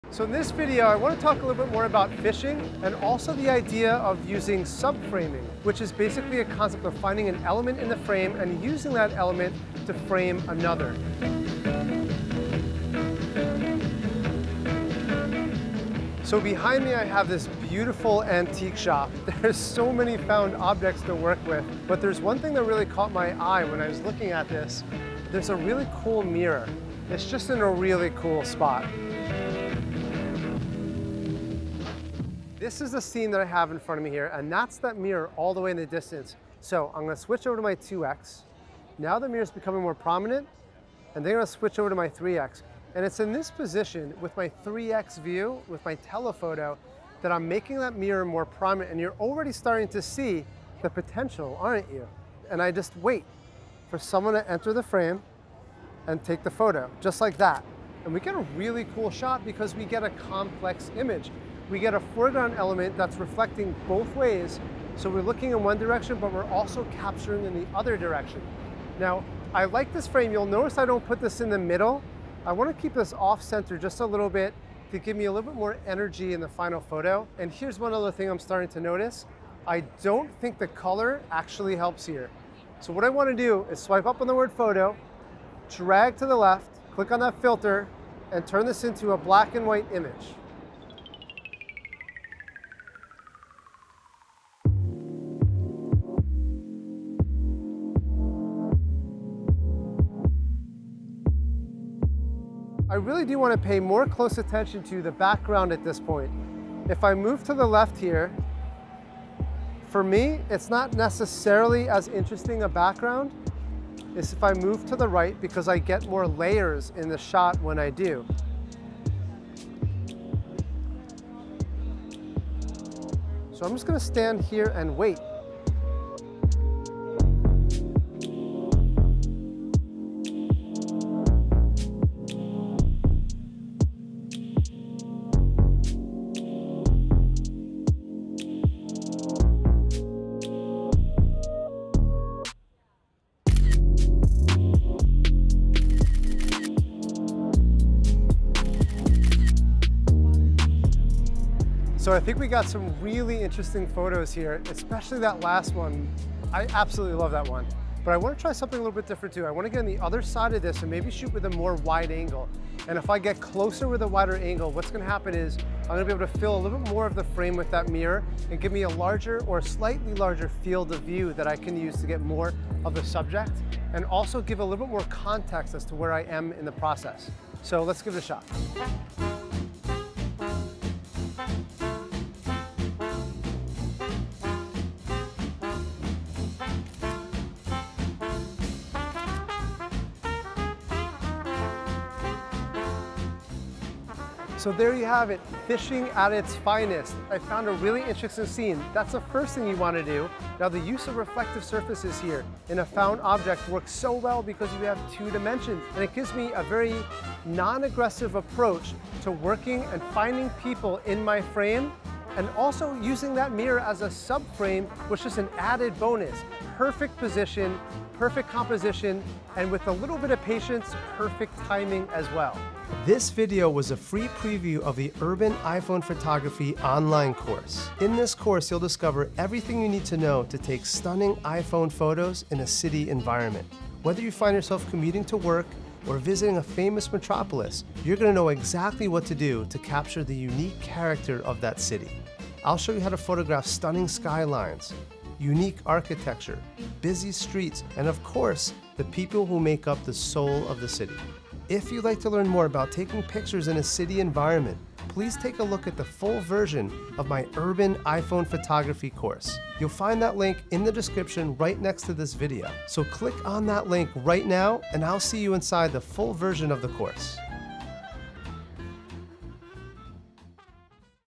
How to Capture Unique iPhone Photos Using Reflective Surfaces
Ready to add a unique artistic touch to your photos?

In this video, we'll show you how to use reflective surfaces to create beautifully complex images.

We'll share incredible composition techniques for using mirrors to frame your subject and get the perfect shot. After watching this video, you’ll be able to capture photos that are full of interesting elements and layers that will leave everyone in awe.

So grab your iPhone and let's get started on taking your iPhone photography to new heights!

Urban iPhone Photography course reveals the secrets to stunning photos in any city!